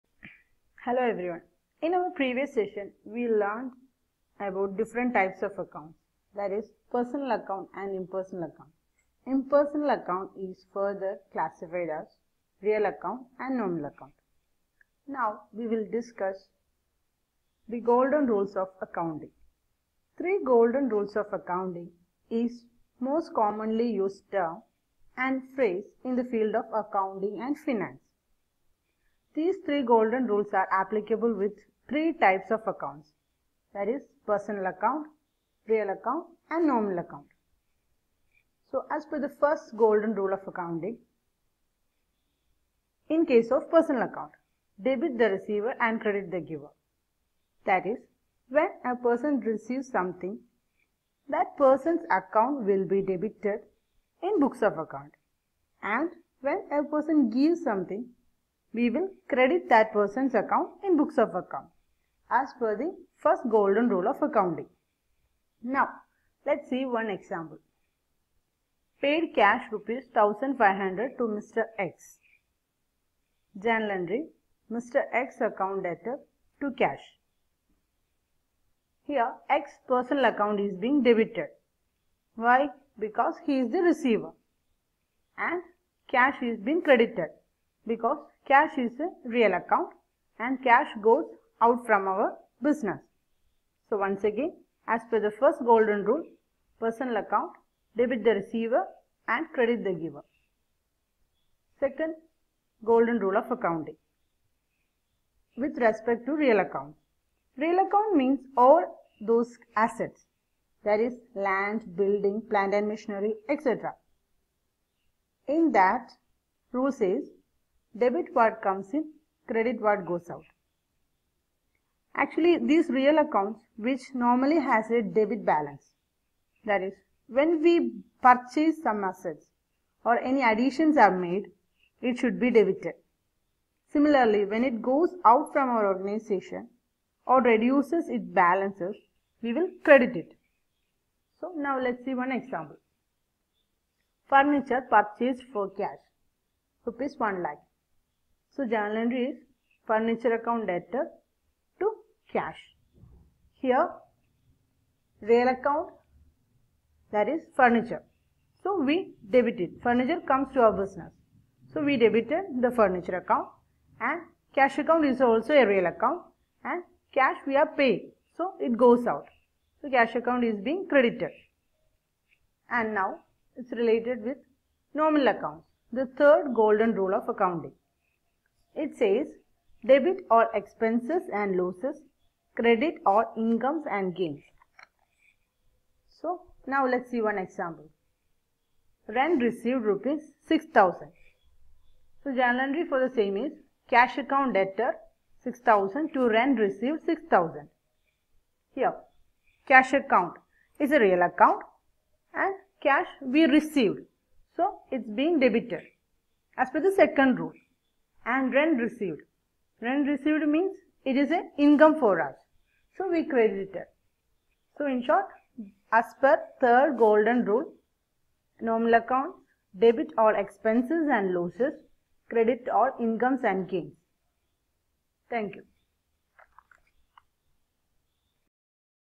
3 GOLDEN RULES OF ACCOUNTING- Personal account,Real account & Nominal account
This video helps to understand the basic rules of accounting and book keeping.

3 Golden rules  of accounting with respect to 3 types of accounts
Personal A/c : Debit the receiver and credit the giver
Real A/c: Debit what comes in and credit what goes out
Nominal A/c: Debit all expenses/losses and credit all incomes and gains.